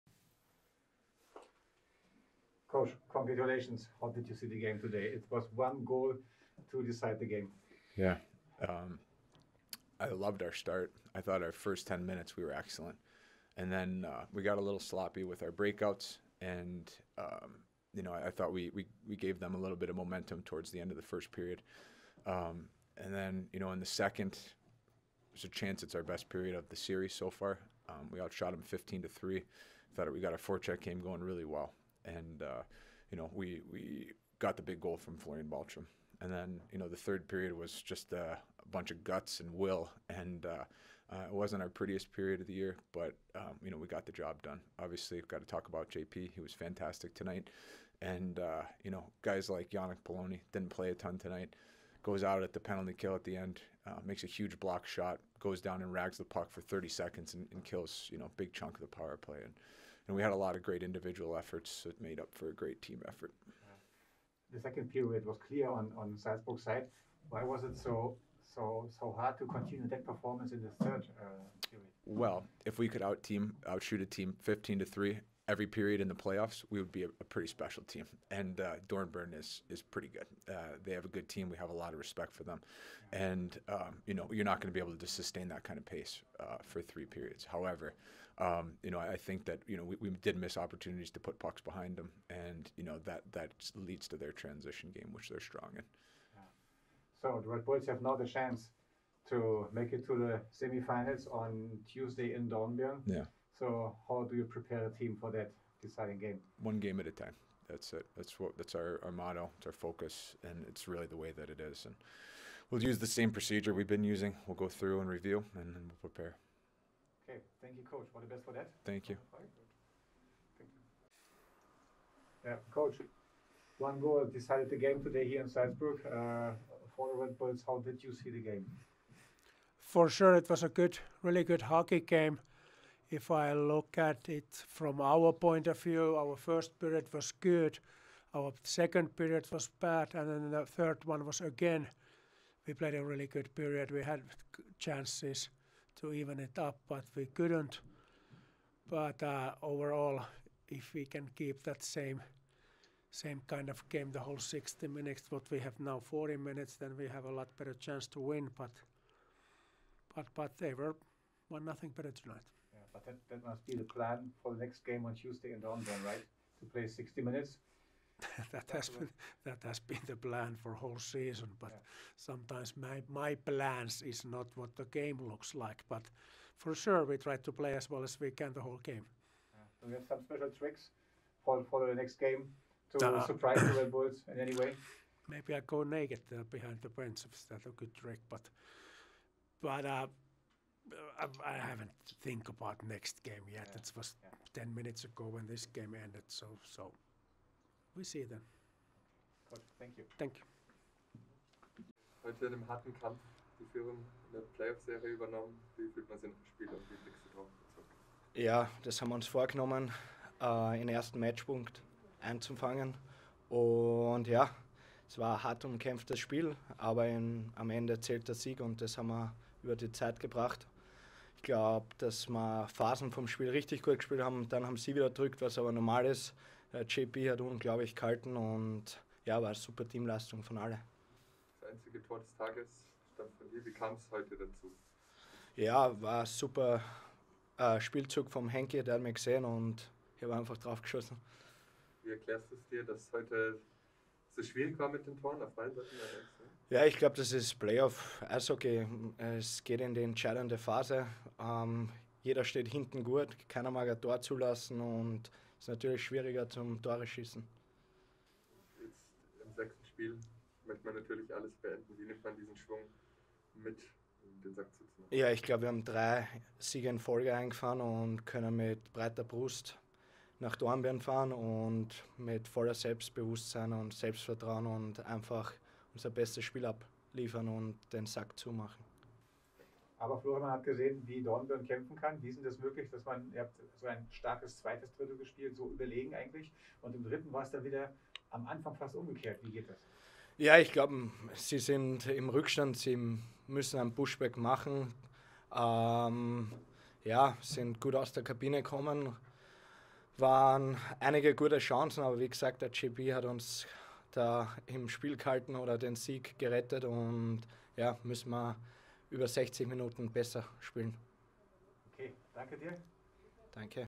Baltram: "Am Ende zählt der Sieg!" | EC Red Bull Salzburg
Nach dem umkämpften 1:0-Sieg der Red Bulls in Spiel5 der Viertelfinal-Serie gegen Dornbirn sprechen Torschütze Florian Baltram, EC Red Bull Salzburg Head Coach Matt McIlvane und Dornbirns Trainer Kai Suikkanen über das Spiel. Sie alle wagen einen Ausblick auf Spiel6 der Serie.

Offizieller Youtube-Kanal des EC Red Bull Salzburg